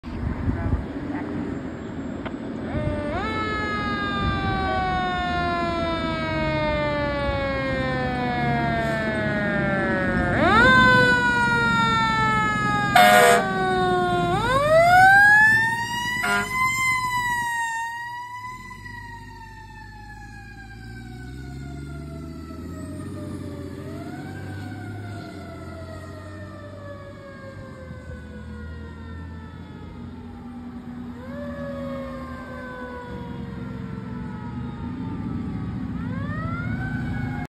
Meriden Fire Department Engine 8 (Engine 1) responding with Q2b and Airhorns!
Meriden Fire Department Engine 1 using their former apparatus responding to an accident with injuries in South Meriden, Connecticut